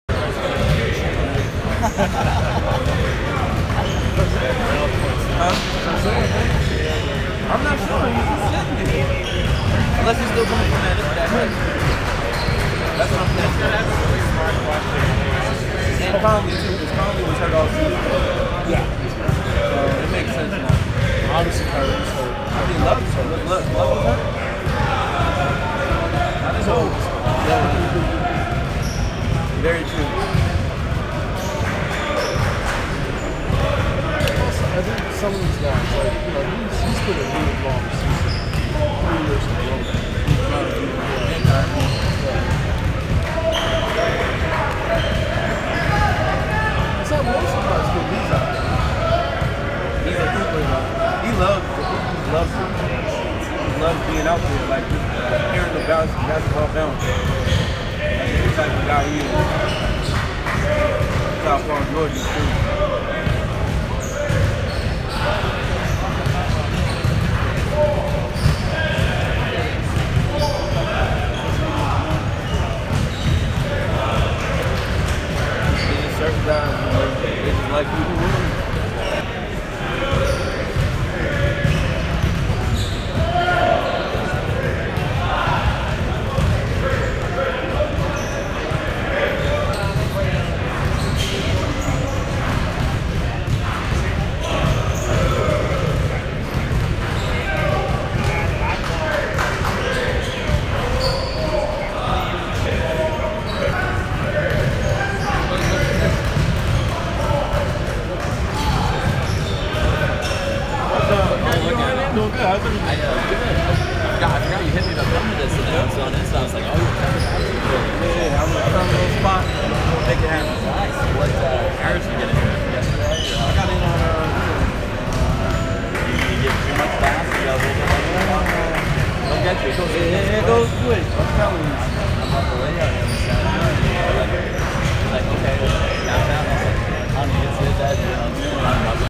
KD (Kevin Durant) in dribble-and-pop drills with Team USA at 2018 USABMNT camp in Vegas
...from USABMNT aka Team USA training camp at Mendenhall Center, UNLV, Las Vegas, NV.